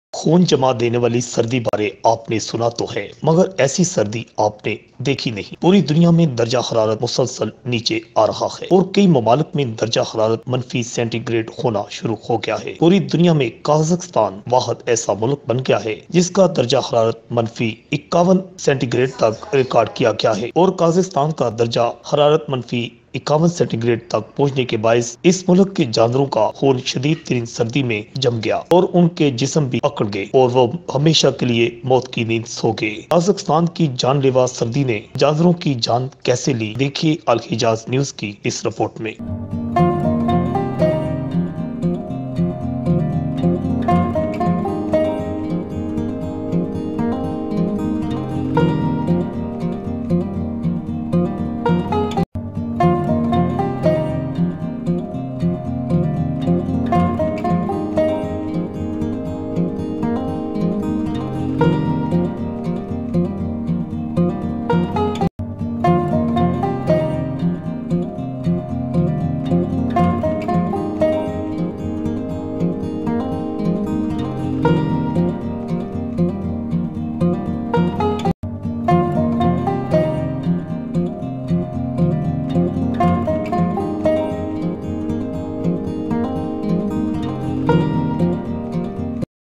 خون جما دینے والی سردی نے جانوروں کی کیسے جان لے لی دیکھئے الحجاز نیوز کی اس رپورٹ میں
خون جما دینے والی سردی بارے آپ نے سنا تو ھے مگر ایسی سردی آپ نے دیکھی نہیں پوری دنیا میں درجہ حرارت مسلسل نیچے آ رھا ھے اور کئی ممالک میں درجہ حرارت منفی سینٹی گریڈ ھونا شروع ھو گیا ھے پورئ دنیا میں قازغستان واحد ایسا ملک بن گیا ھے جس کا درجہ حرارت منفی 51 تک ریکارڈ کیا گیا ھے اور قازغستان کا درجہ منفی 51 سینٹی گریڈ تک پہنچنے کے باعث اس ملک کے جانوروں کا خون  شدید ترین سردی میں جم گیا اور ان کے جسم بھی اکڑ گئے قازقستان کی جان لیوا سردی نے جانوروں کی کیسے جان لے لی دیکھئے الحجاز نیوز کی اس رپورٹ میں